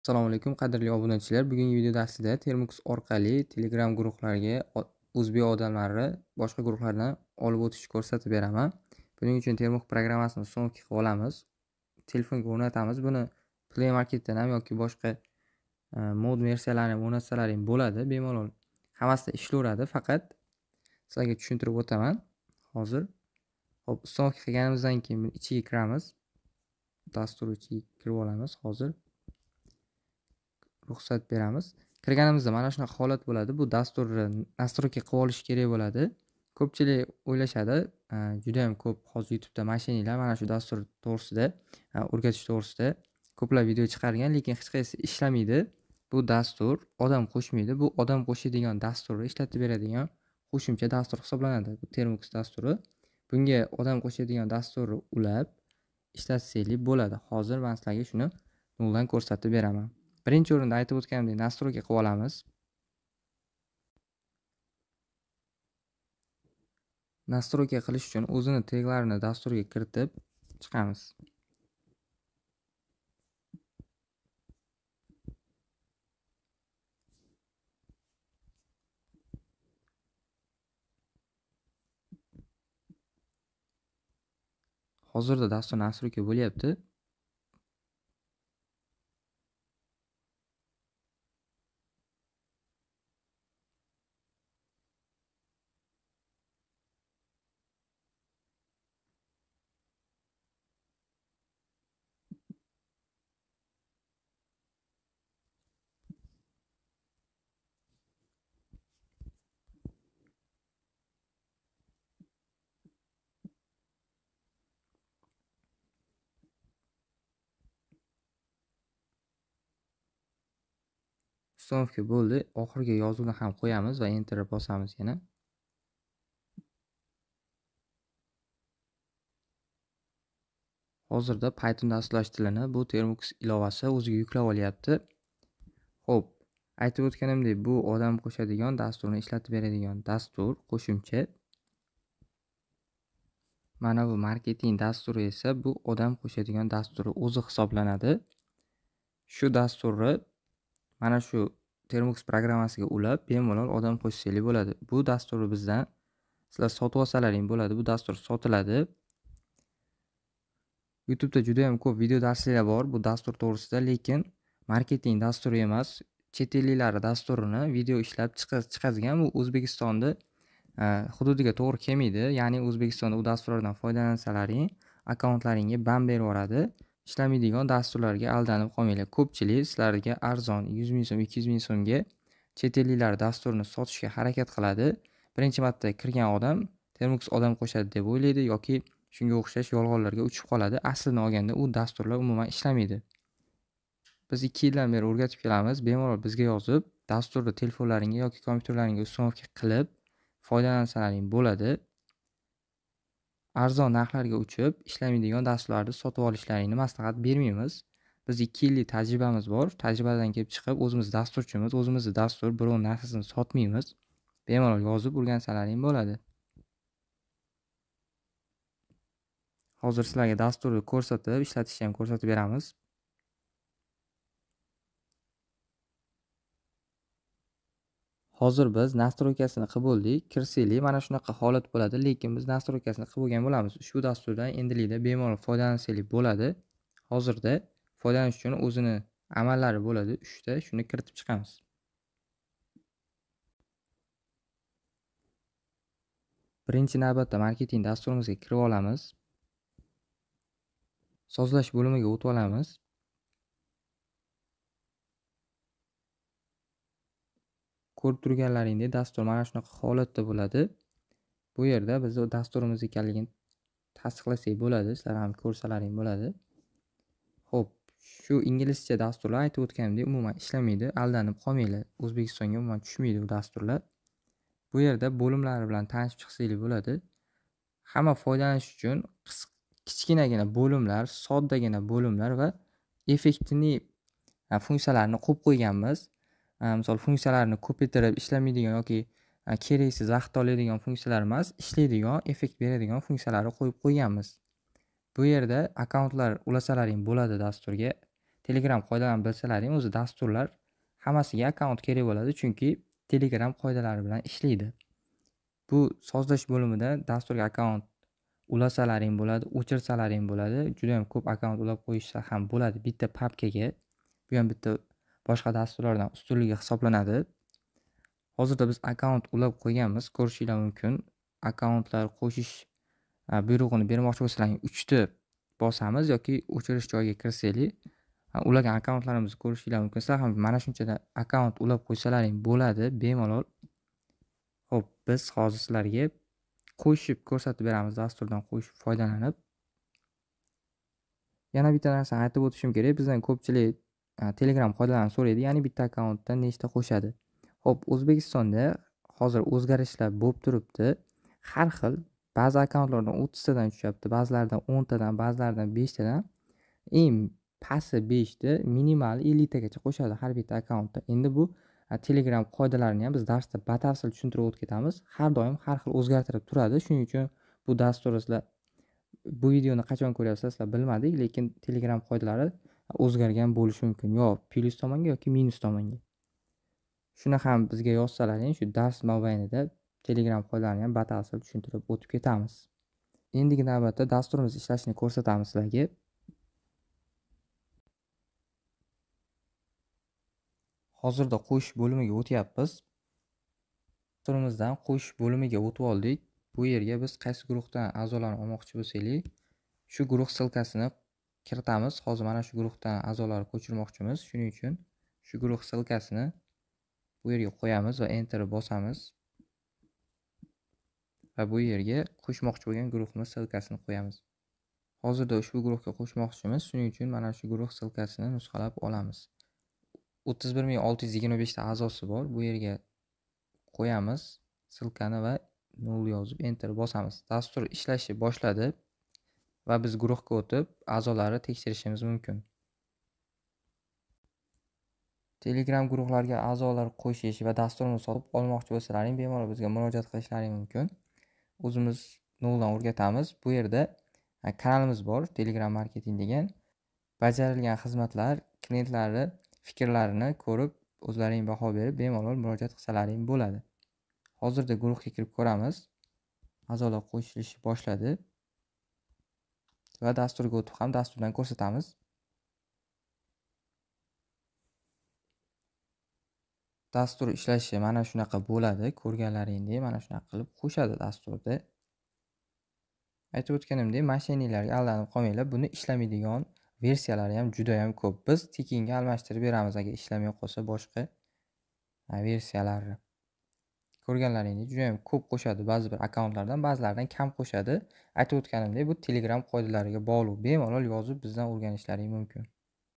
Termux orqali telegram guruhlarga uzbek azolar qoshish yangi usuli | New 2023!
Telegram marketing bilan 2 yildan buyon shug'ullanib hozirda 1000dan ko'p o'quvchilarga erishdik va ulrani ko'pchiligi hozirda yaxshi natijalarga erishib kelmoqda!